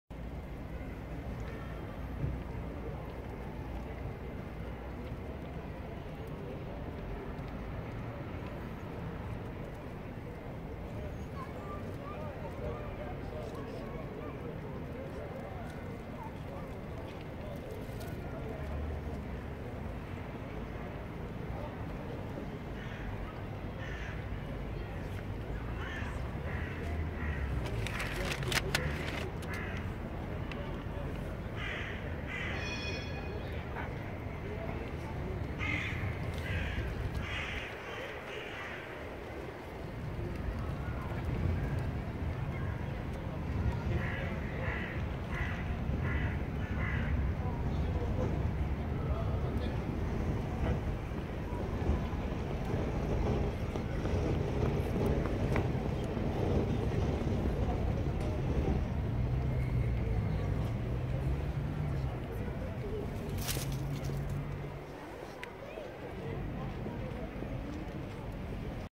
Hamster am Reumannplatz
da raschelts auf einmal in einem der hochbeete und ich halt ausschau nach einer ratte oder maus. auf einmal klettert der hamster auf die pflanzen rauf.

1.10.17